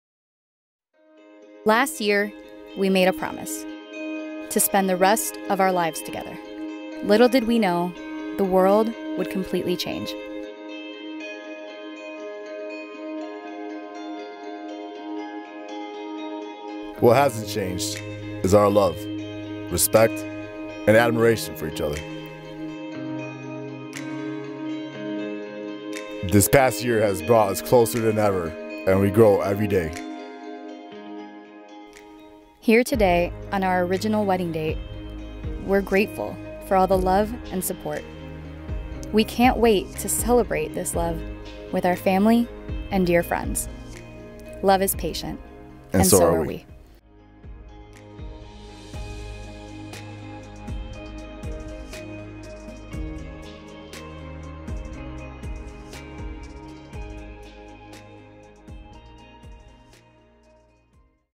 Arbella + Danny
Due to Covid, Arbella and Danny had to move their wedding back to 2022. They wanted to make a video to exhibit their love to their friends and family! We can't wait for their big day!